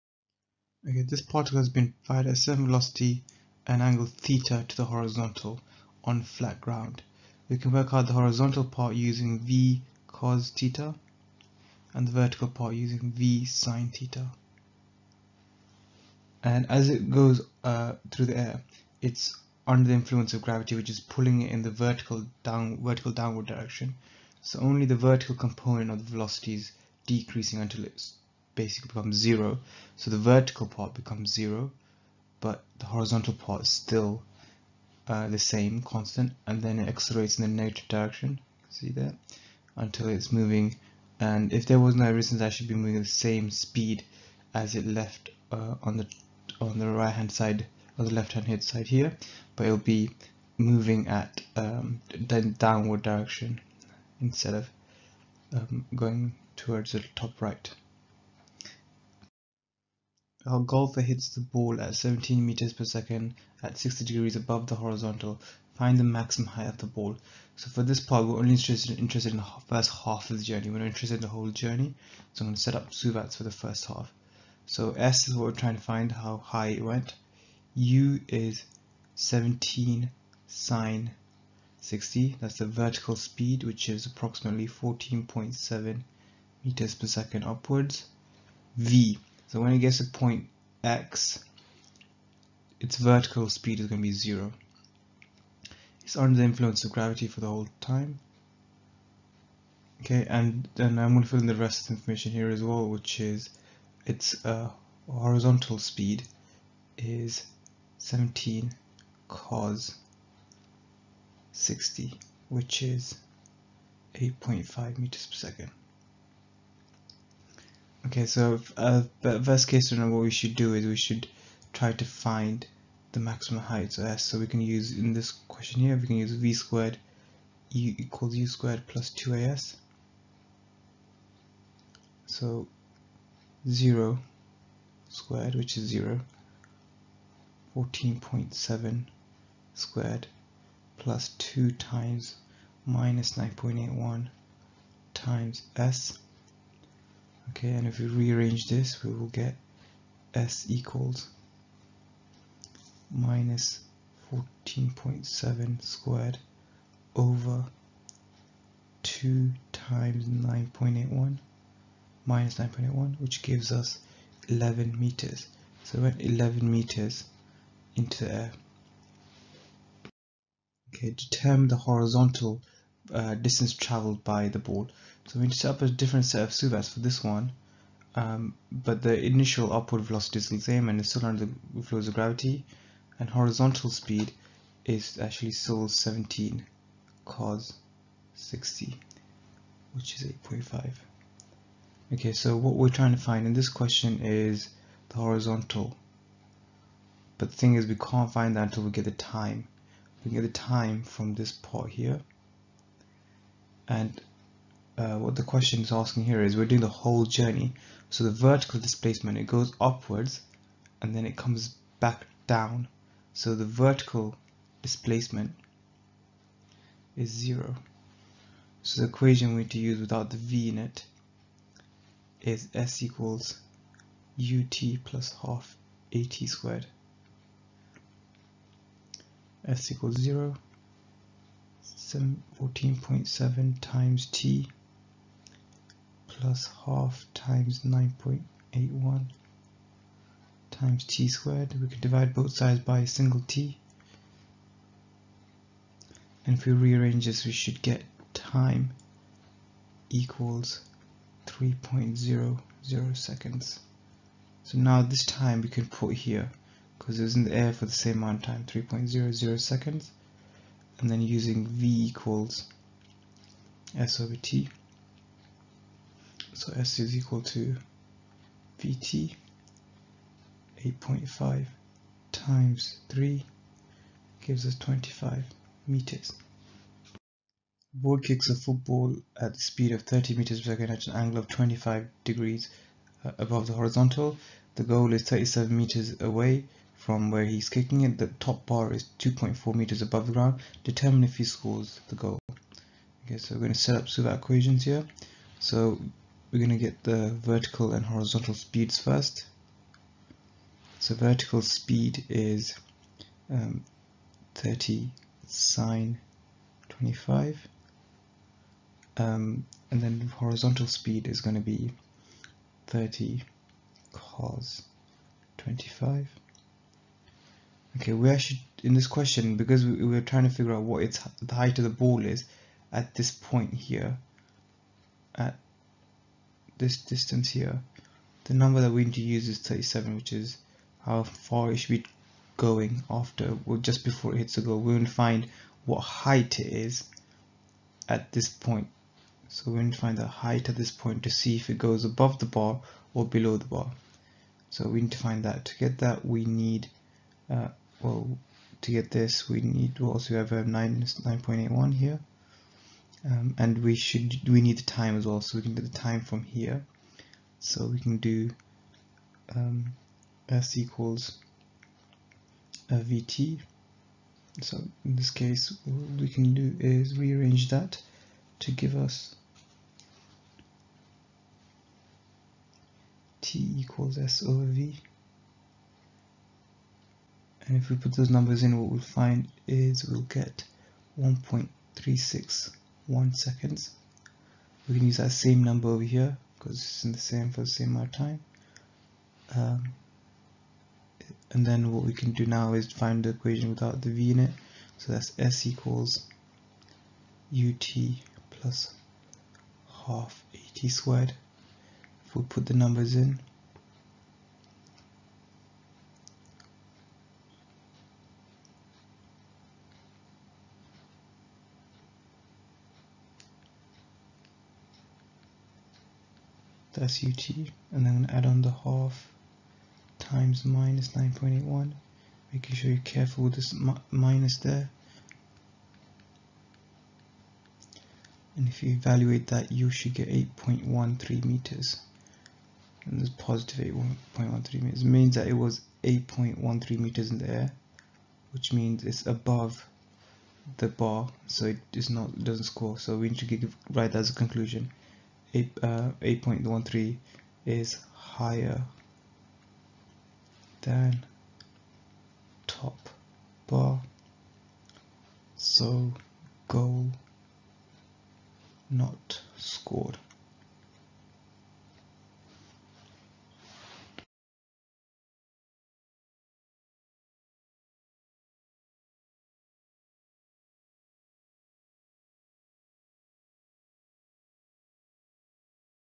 1.14 Projected diagonally on flat ground
This Video covers the mechanics topic from A-level Physics/ AS Physics for the AQA specification.